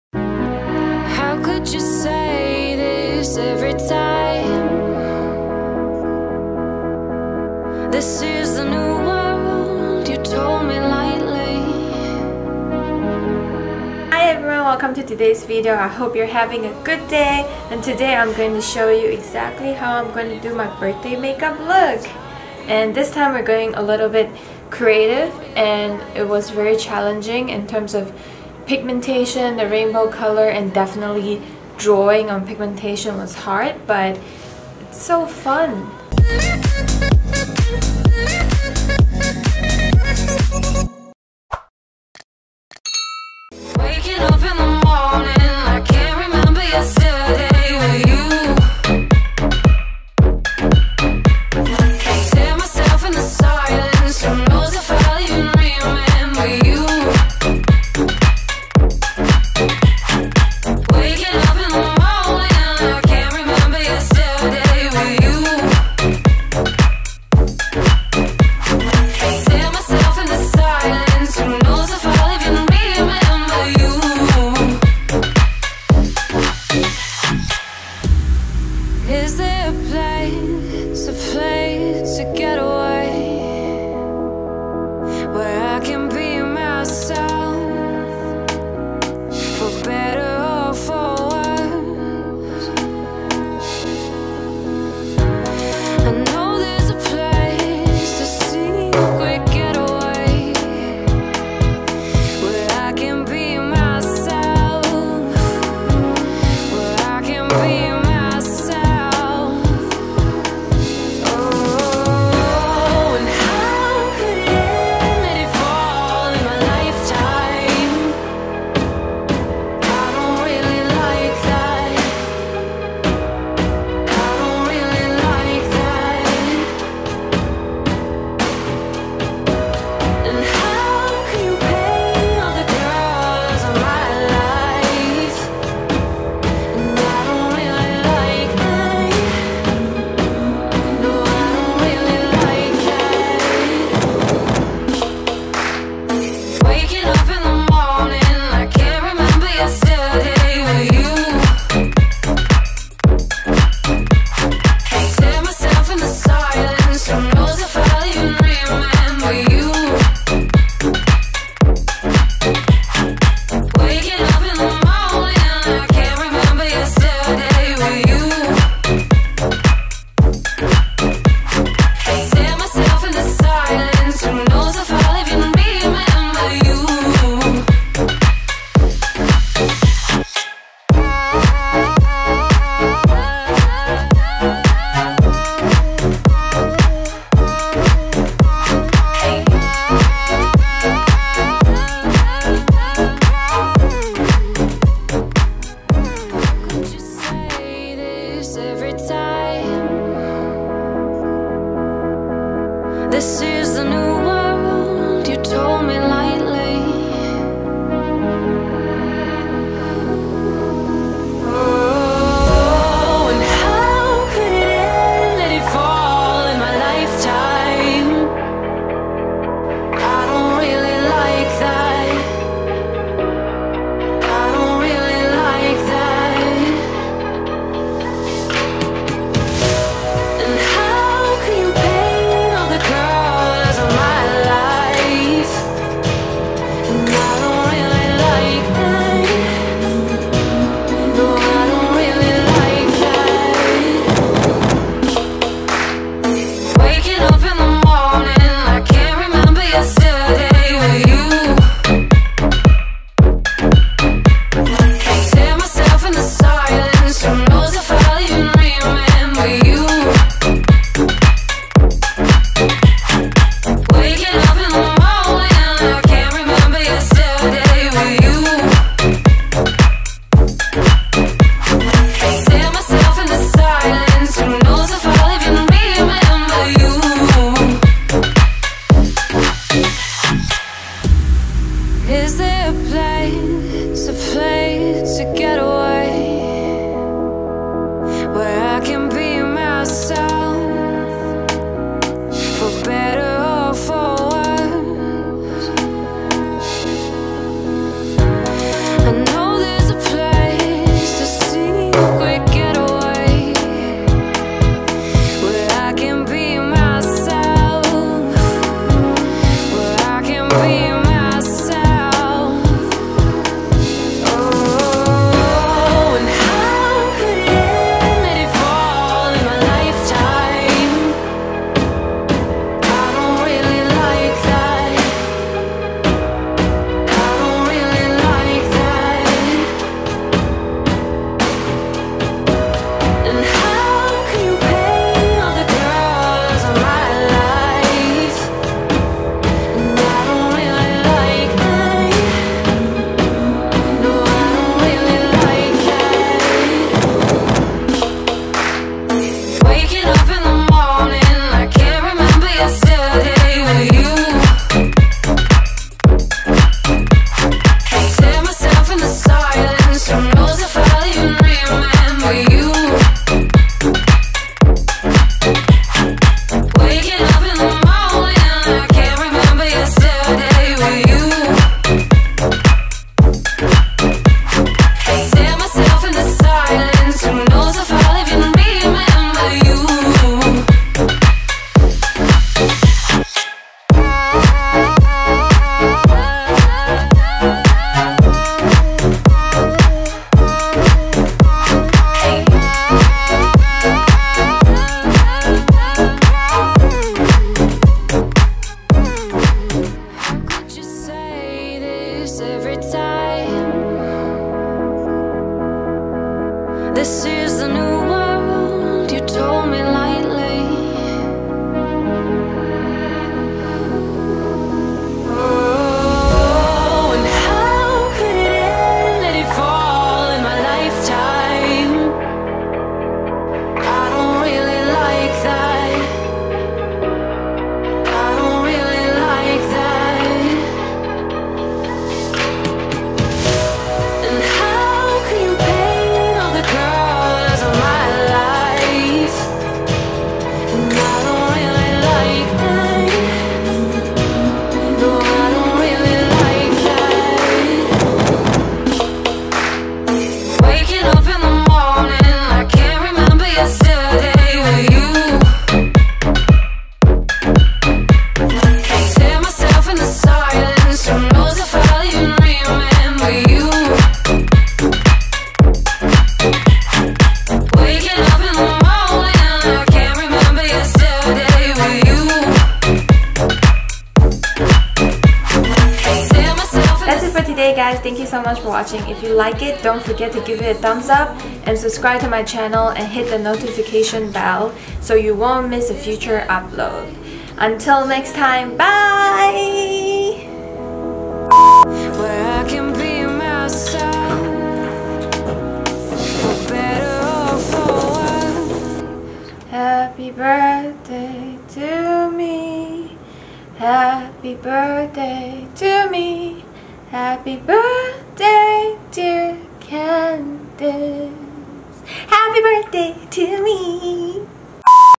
CREATIVE BIRTHDAY MAKEUP 2018 | Candice Artely
🎂 CREATIVE BIRTHDAY MAKEUP TUTORIAL 2018 🎈

Hiiiii guys! I know is suppose to be Christmas season, and everyone is doing Christmas look, but too bad, I was born in December so its more than just Christmas season for me, it is also BIRTHDAY for me! Hence, I decided to do this look and challenge myself a little. As usual, let me know what you guys think! And of cause thank you sooooo much for watching! Love, xoxo.

⭐️PRODUCTS MENTIONED⭐️
P.Louise Base Shade 0
Juvia’s Place Masquerade Palette
Kryolan Aqua Color K Palette 24 colors
Too Faced Born This Way Super Coverage
Jeffree Star Cosmetics Velour Liquid Lipstick Shade Drug Lord
K Palette 1 Day Tattoo Eyeliner
LashyliciousLashes Foxylicious
Litcosmetics Litmetal Glisten, Cayman & Afternoon Delight
Anastasia Beverly Hills Powder Contour Kit
Hourglass Ambient Strobe Lighting Blush - Euphoric Fusion
Ofra Glazed Donut Highlighter
M.A.C. Extended Play Gigablack Lash
Anastasia Beverly Hills Lipstick
Marc Jacob Beauty Lipgloss